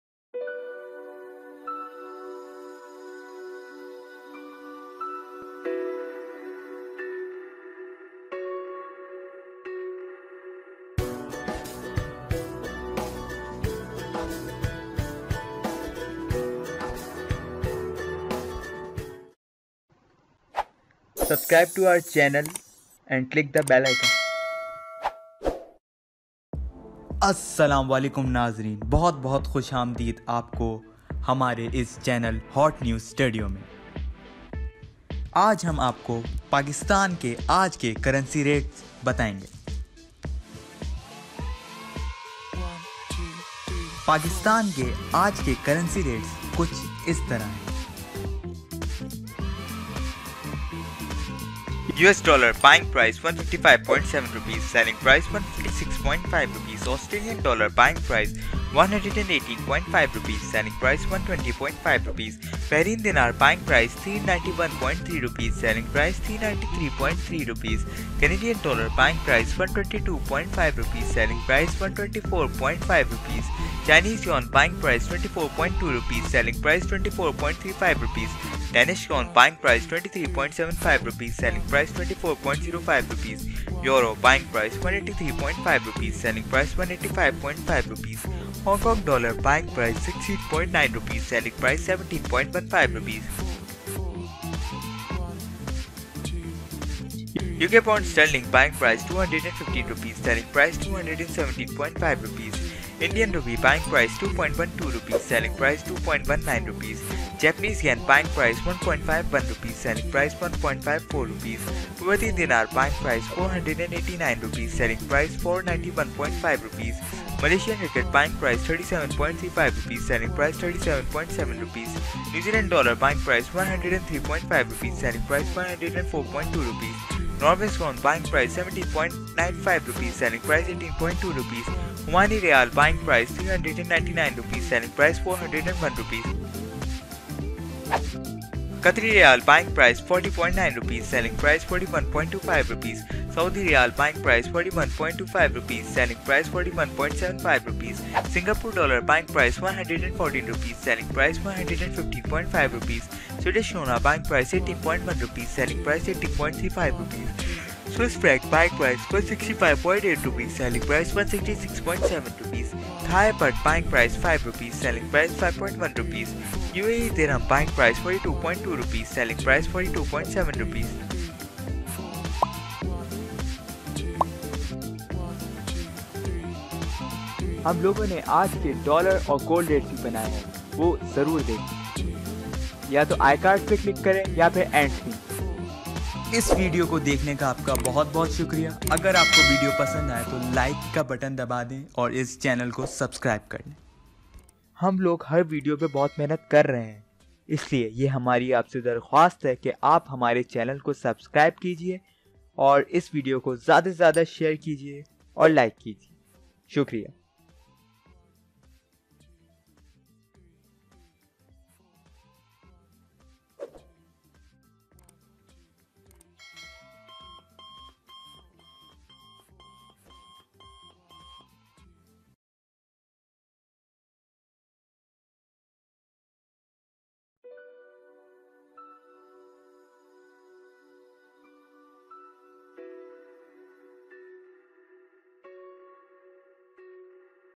Today's Open Market Currency Rates in Pakistan |PKR Exchange Rate| 22nd March 2021
Hey Viewers,
          We are back with our segment " Economics". In this video we will tell you guys todays OPEN MARKET RATES / currency rates of PAKISTAN.
In this video we have showed today's PAKISTAN'S CURRENCY RATES.

Green Screens
Instagram Green Screen Videos — No Copyright)

Facebook Green Screen (Footage green screen and Background Videos)

Following Search terms are covered in this video.